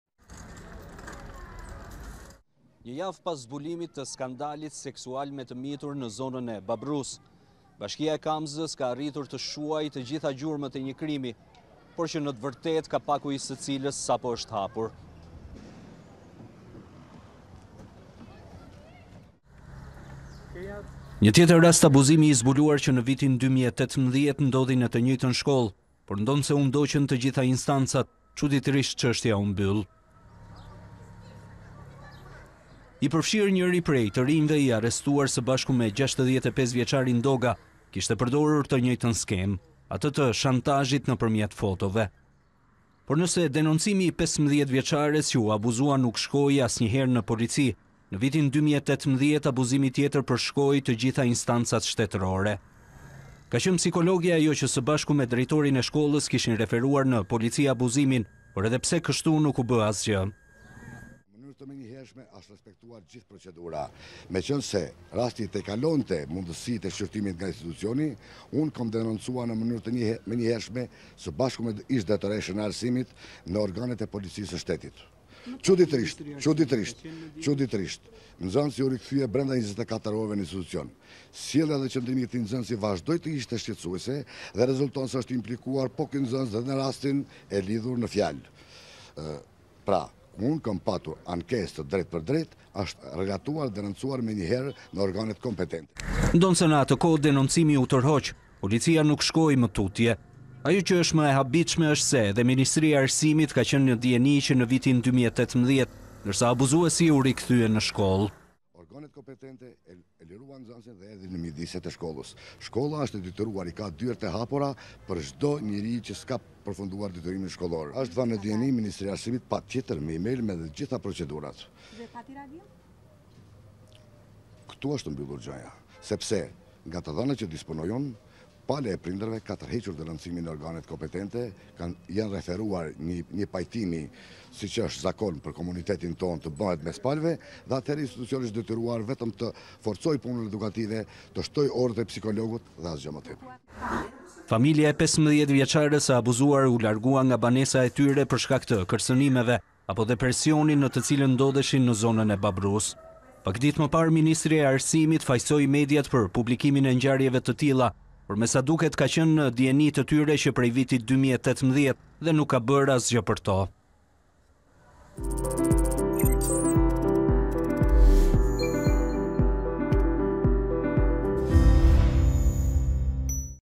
Tjetër histori abuzimi në Babrru, Ministria e Arsimit dhe Policia ishin në dijeni që në vitin 2018
Një tjetër rast abuzimi i zbuluar që në vitin 2018 ndodhi në të njëjtën shkollë por ndonëse u ndoqën të gjitha instancat çuditërisht u mbyll. i përfshirë njëri prej të rinjve i arrestuar së bashku me 65-vjeçarin Doga kishte përdorur të njëjtën skemë. Atë të shantazhit nëpërmjet fotove. Por nëse denoncimi i 15-vjeçares që u abuzua nuk shkoi asnjëherë në polici në vitin 2018 abuzimi tjetër përshkoi të gjitha instancat.